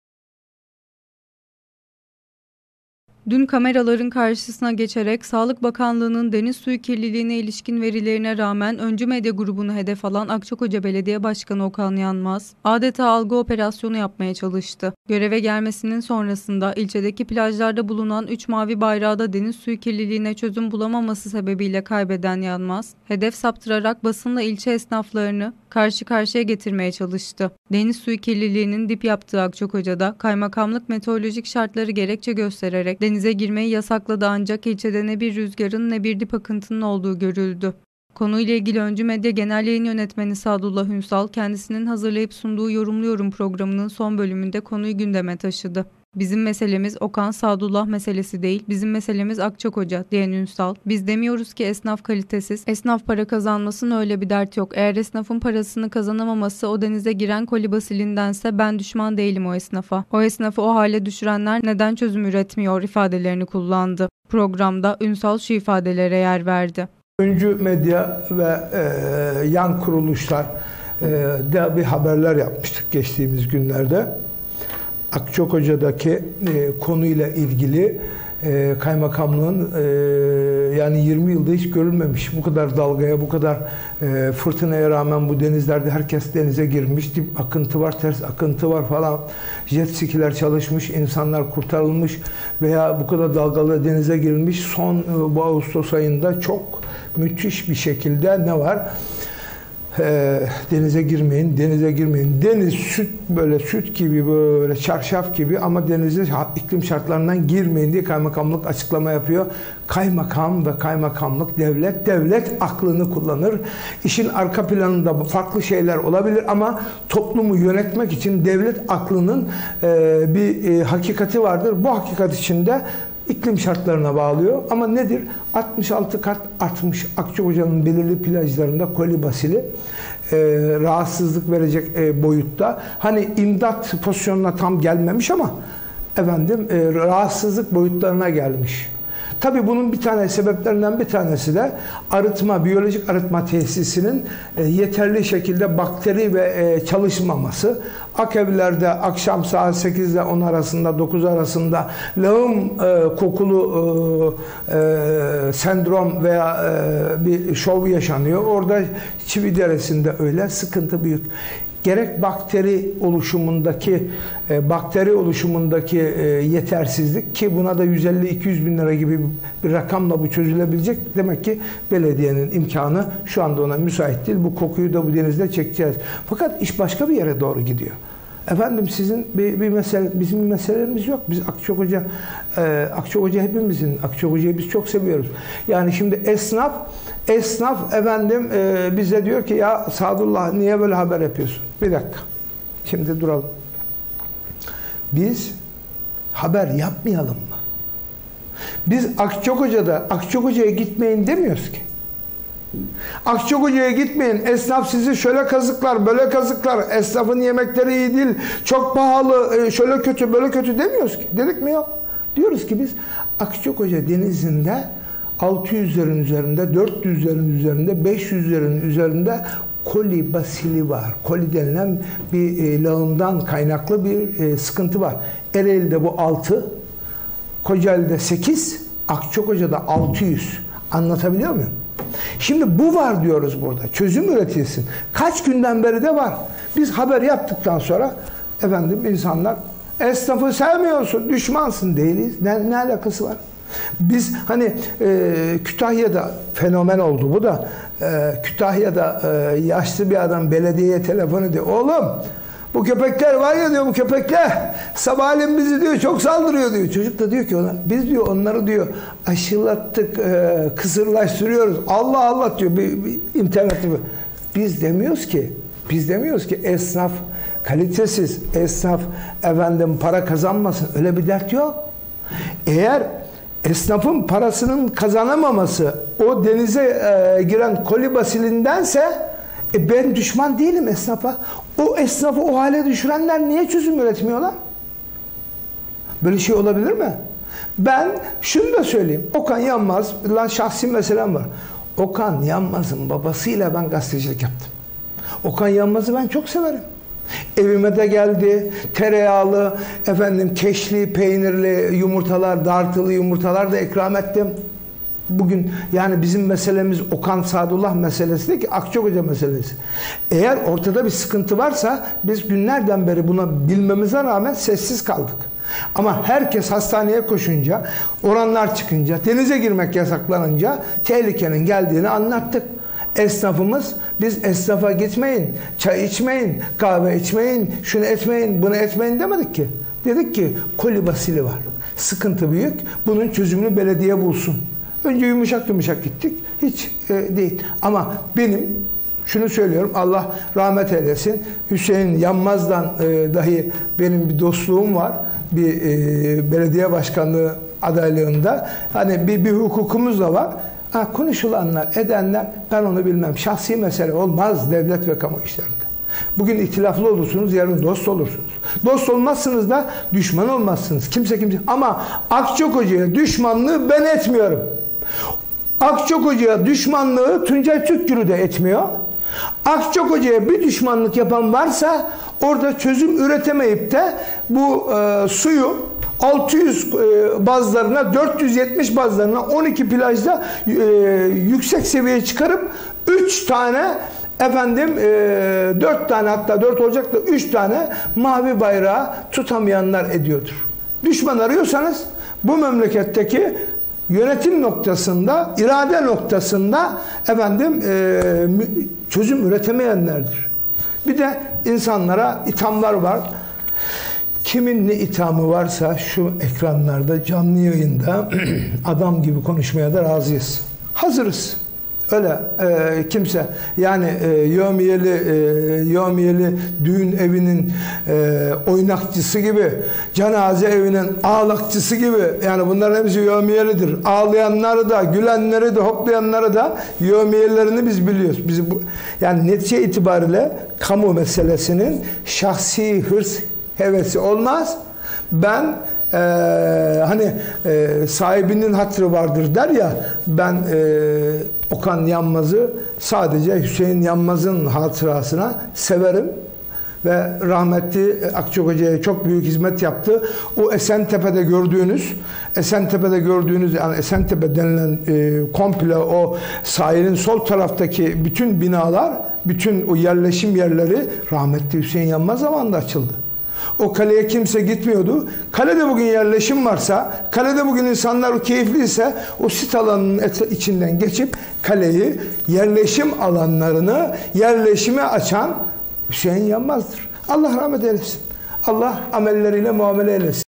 ÖNCÜ MEDYA HALKIN YANINDA! ASIL AKÇAKOCA DÜŞMANI KİRLİLİĞİ GÖRÜP ÇÖZMEYENDİR
Akçakoca’nın sadece plajlarını değil kanalizasyon sularının yeterli arıtılmaması sonucu denizini de esir alan kirliliğin gündeme bomba gibi düşmesinin ardından adeta köşeye sıkışan Belediye Başkanı Okan Yanmaz, Sağlık Bakanlığı verileri ortadayken, Öncü Medya’ya ‘Akçakoca düşmanlığı’ iftirası atmaya kalktı. Kendisi de Akçakocalı olan ve memleketinde doğru iş yapana ‘Helal olsun’ dediği gibi yanlışa da ‘Durun!’ diyerek ses yükselten Öncü Medya Genel Yayın Yönetmeni Sadullah Ünsal, “Bizim meselemiz Okan-Sadullah meselesi değil. Bizim meselemiz Akçakoca!” diyerek Yanmaz’ın basını hedef göstermesini eleştirdi. Ünsal ayrıca, “Biz ‘Akçakoca esnafına gitmeyin.’ demiyoruz ki… Biz, ‘Denizde koli basil var, sıkıntı büyük, bunun çözümünü belediye bulsun.” diyoruz. Akçakoca’ya düşmanlığı 3 mavi bayrağı tutamayanlar ediyordur.” sözleri ile Öncü Haber’in Akçakocalılar’ın yanında olduğunu vurguladı.